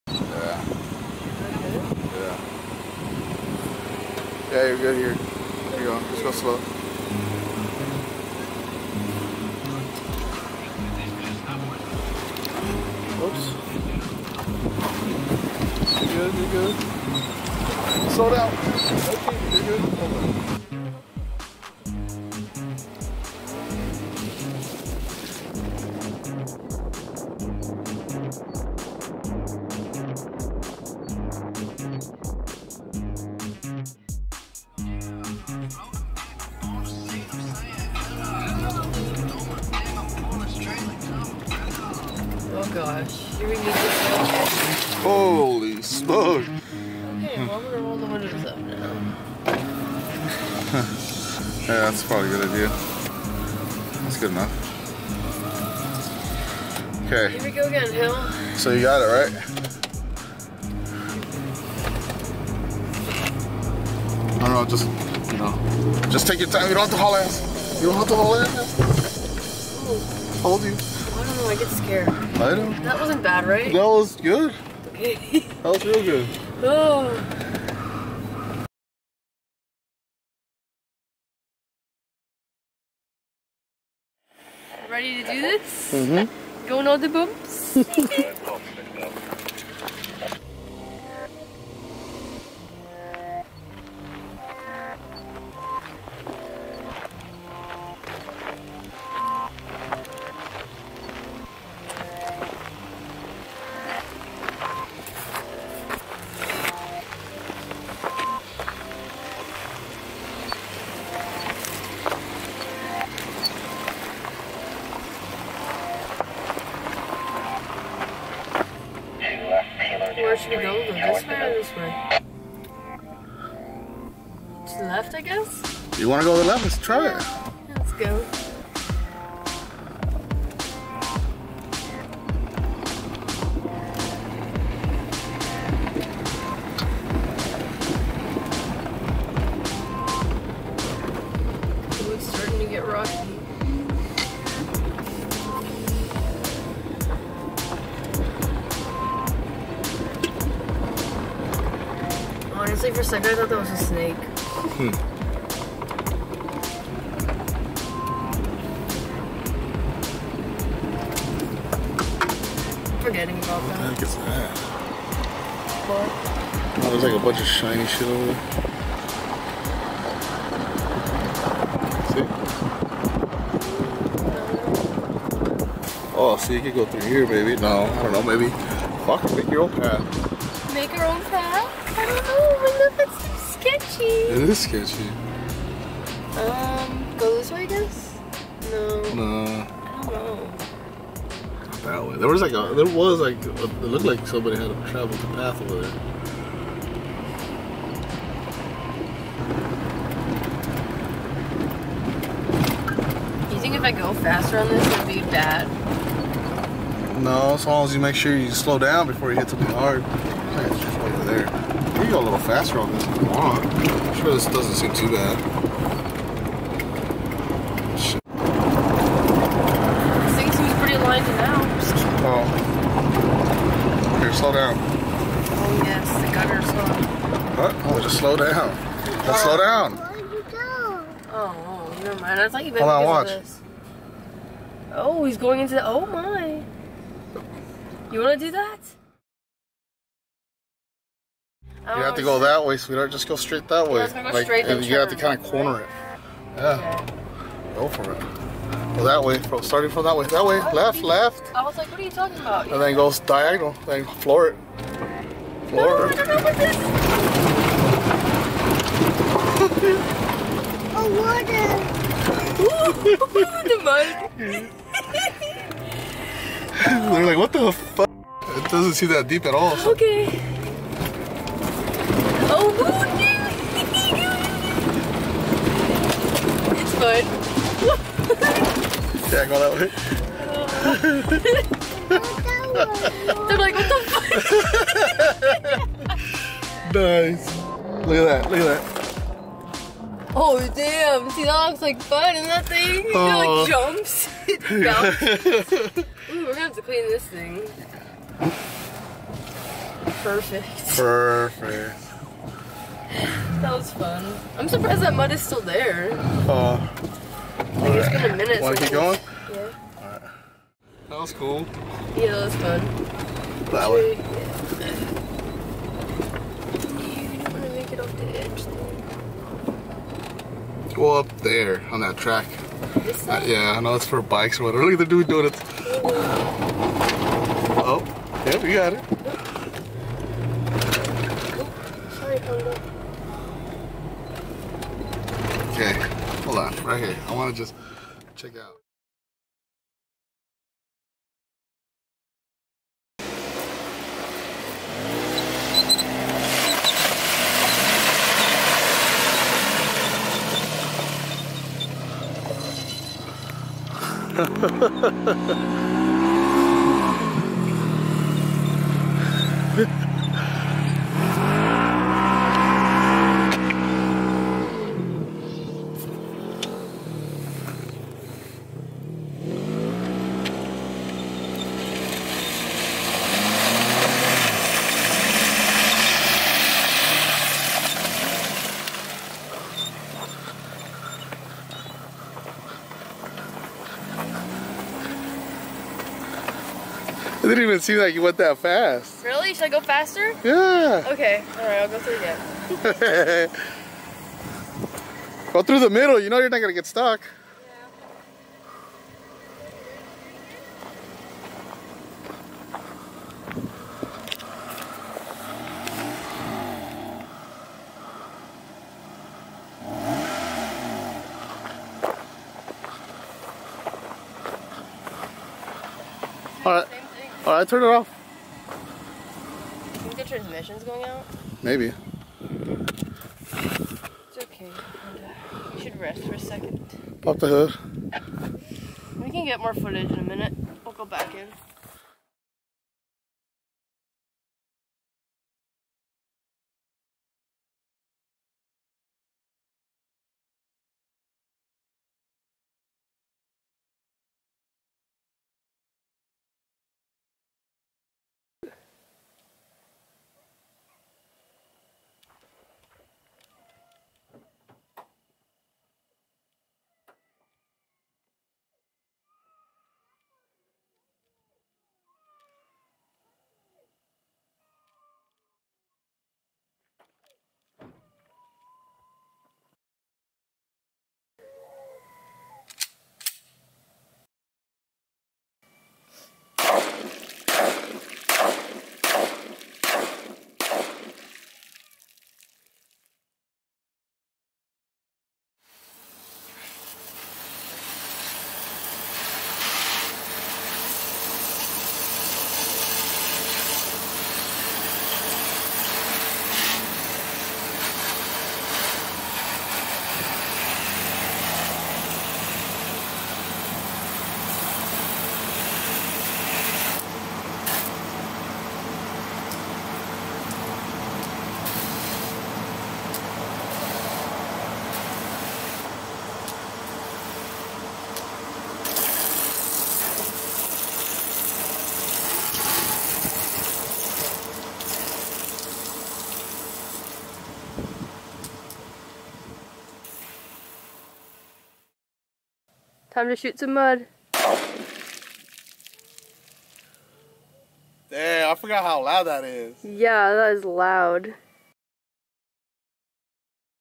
RD1 Dead Transmission
Hello again, and welcome to another Valencia Family Adventure. In this video we go through some more mud and take the RD1 to a new place; to explore new terrain. We went to a place with more water and dirt and got into a muddy mess. The CR-V runs into transmission problems and barely makes it back home. What will happen to the RD1 in the next episode?